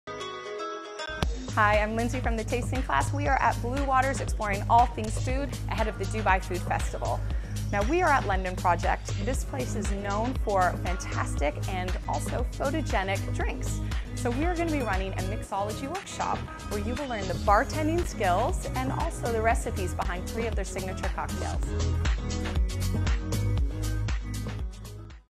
The Tasting Class Bluewaters Takeover for Dubai Food Festival - London Project
The London Project is famous for its tasty, photogenic, over-the-top cocktails. As part of our Dubai Food Festival, Bluewaters Island takeover we rolled up your sleeves and made a few of them! As we built three of their signature cocktails, attendees learned how to mix, shake, strain, and muddle like a pro, while also learning about the products in these historically inspired drinks. All served with a selection of nibbles throughout the experience.